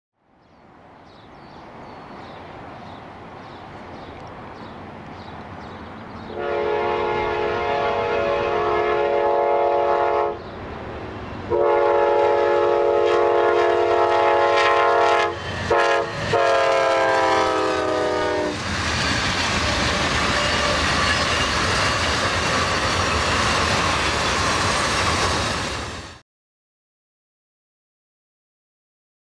Amtrak 145 6-21-11
Amtrak heritage unit 145 on train #6 at West Causeway, between Davis and Sacramento 0n 6/21/11.